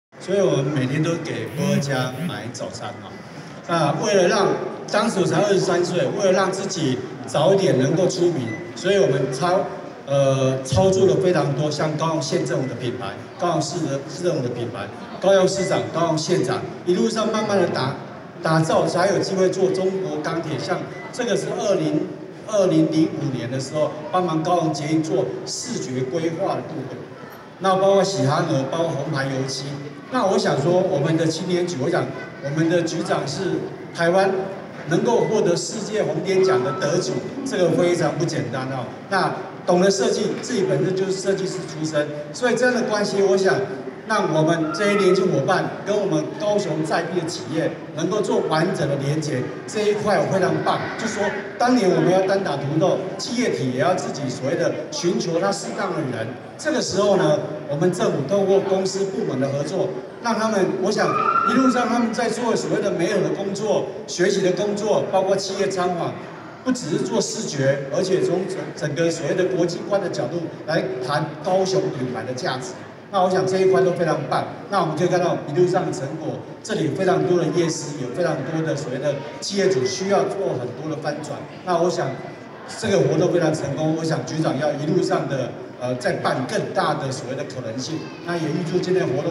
亞洲全球新聞（記者胡黎云，陳文娟/高雄青年局報導）2024大港經典升級提案競賽 第一名獎金20萬元
高雄市政府青年局在113年10月12日下午三點
高雄美麗島站光之穹頂隆重舉辦，青年設計專
業團隊盡情揮灑創意設計，為多家經典品牌注
入全新生命。（志斌食品）袁偉菁經理對參賽
團隊的設計給予高度讚賞，精益求精態度，認
真雕琢每個細節，外豐沛創意與設計實力成功將企業經典與現代美學結合，成果令人驚豔，
期待未來將作品能付諸實現提升品牌形象。

下方可留言，分享，訂閲